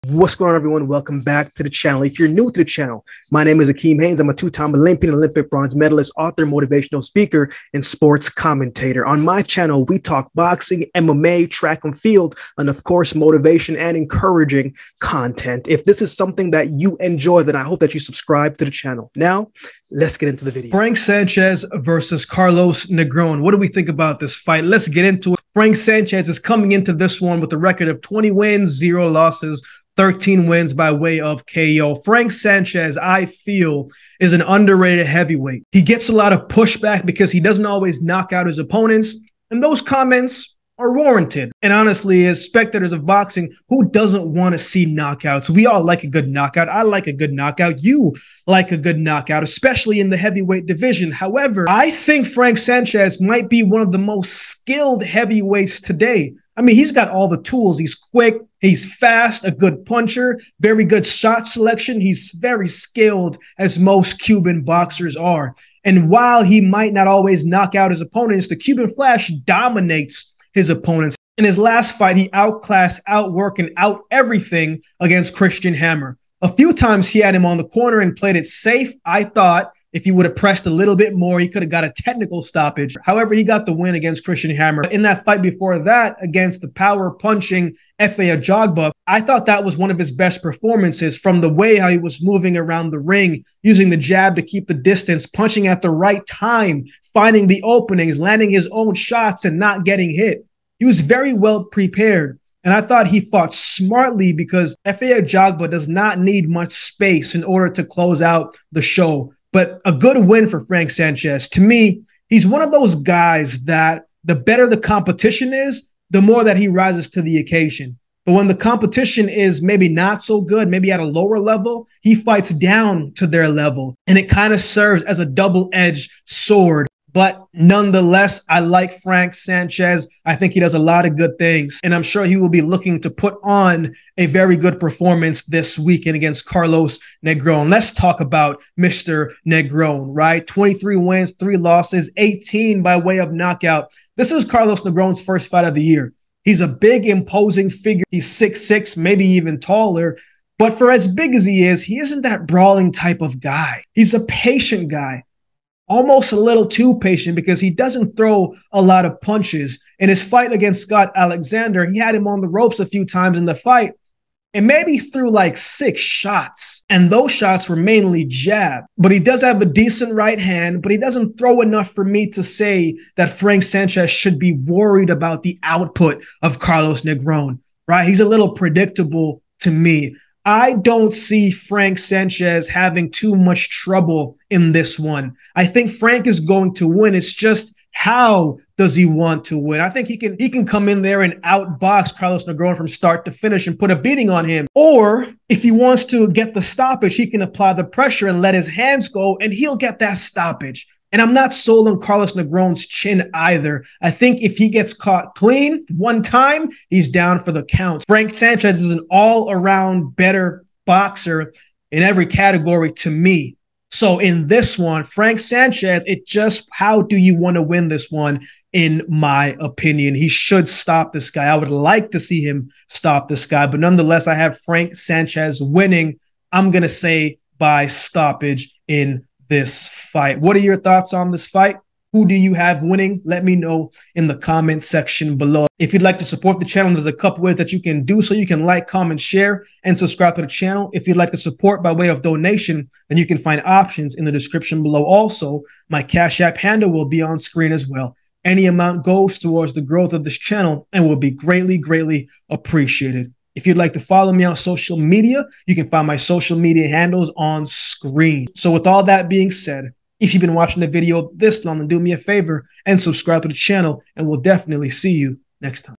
Frank Sanchez Vs Carlos Negron Prediction, Who Wins?
The Cuban flash frank Sanchez is back in the ring this weekend against an imposing heavyweight in Carlos Negron. I dont think Sanchez should have too much trouble in this one.. but let's talk about it.. how does it go?

Or

Your support goes a long way in helping the growth of the channel.

Much love!